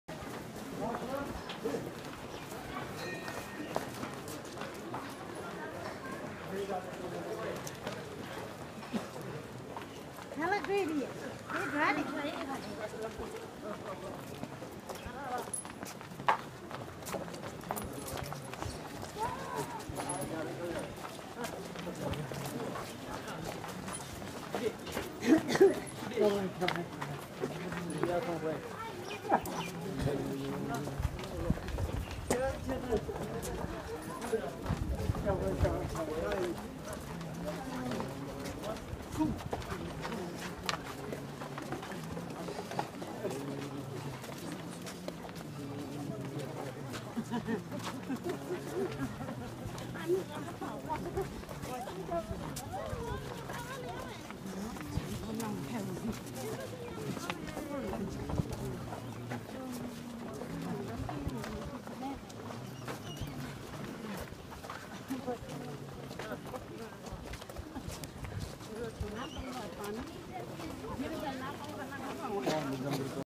1 round of Rewalsar lake with rinpoche , Mandi Himachal Pradesh
1 round of Rewalsar lake with rinpoche , Mandi Himachal Pradesh